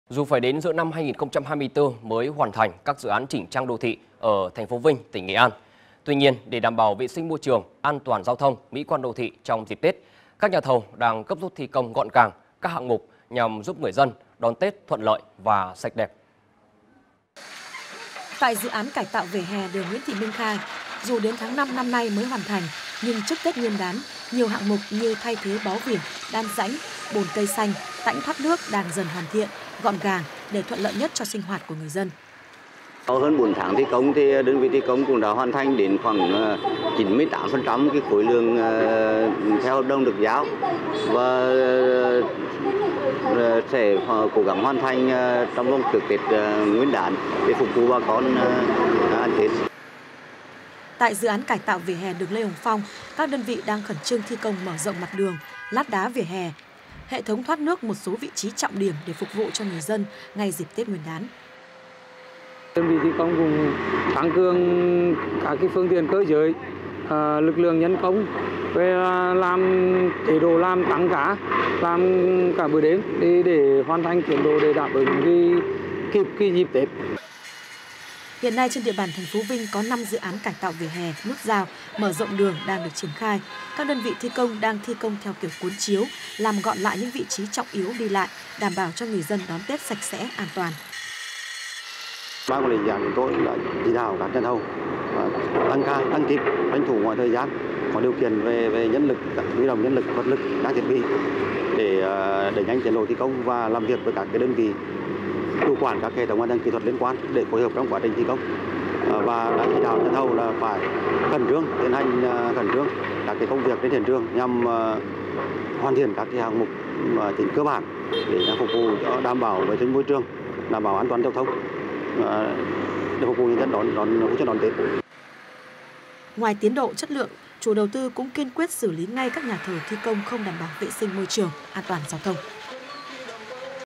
Nghệ An gấp rút thi công, đẩy nhanh chỉnh trang đô thị trước Tết Nguyên đán | VTC1
VTC1 | Nghệ An gấp rút thi công, đẩy nhanh chỉnh trang đô thị trước Tết Nguyên đán
Dù phải đến giữa năm 2024 mới hoàn thành các dự án chỉnh trang đô thị ở Thành phố Vinh, tỉnh Nghệ An. Tuy nhiên, để đảm bảo vệ sinh môi trường, an toàn giao thông, mỹ quan đô thị trong dịp tết, các nhà thầu đang gấp rút thi công gọn gàng các hạng mục nhằm giúp người dân đón Tết thuận lợi và sạch đẹp.